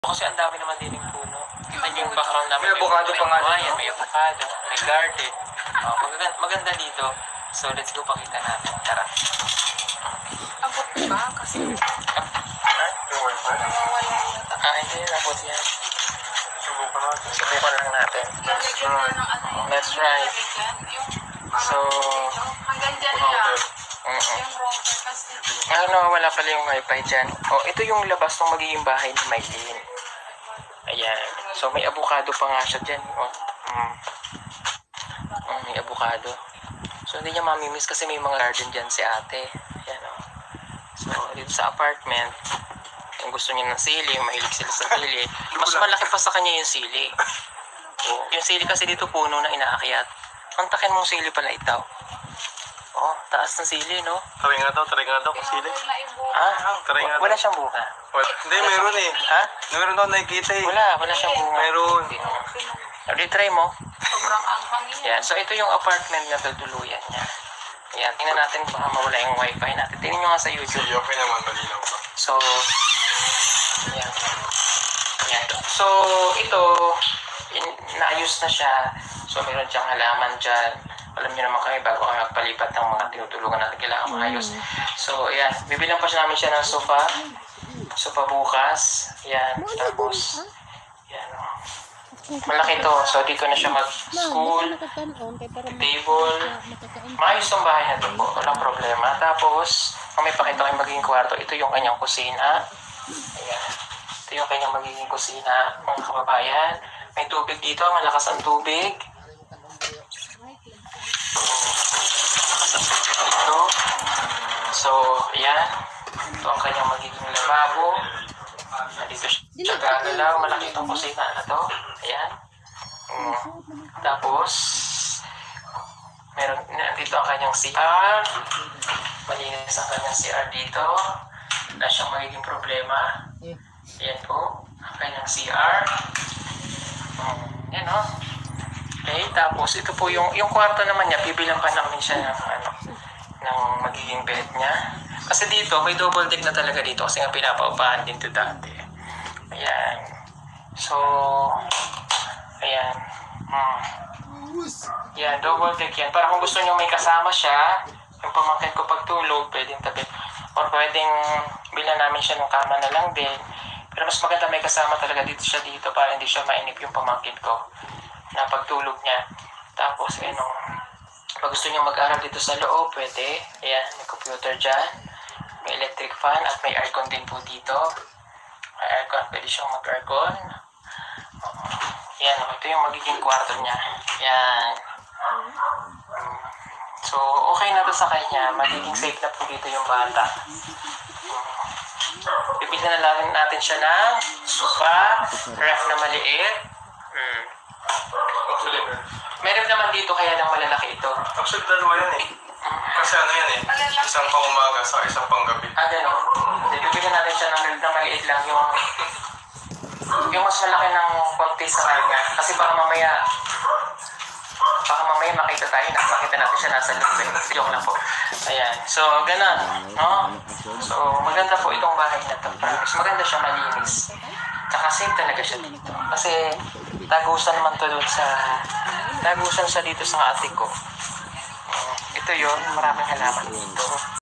Hala!Bonggang bahay para Kay maylene pinasilip ni "Pugong Byahero"pugongbyahero
We love you all. Salamat sir Pugong Byahero sa tulong mo para Kay mylene.